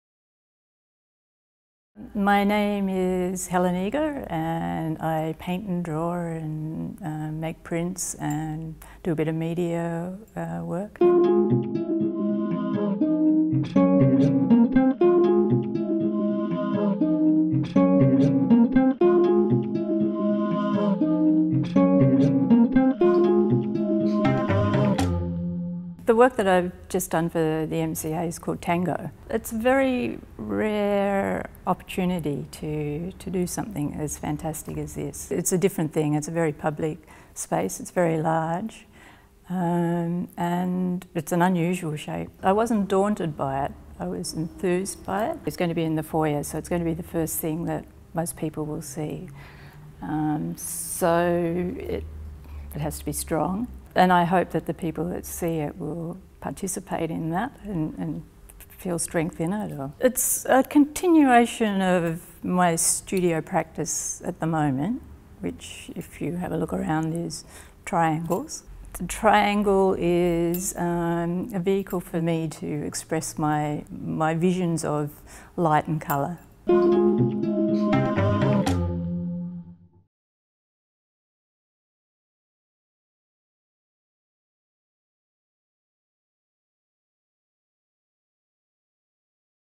Helen Eager Introduces 'Tango' (MCA 2012 Commission)
Helen Eager talk about her practice and her work TANGO.

TANGO was commisioned by the Museum of Contemporary Art, on the occasion of the opening of the new MCA, March 2012.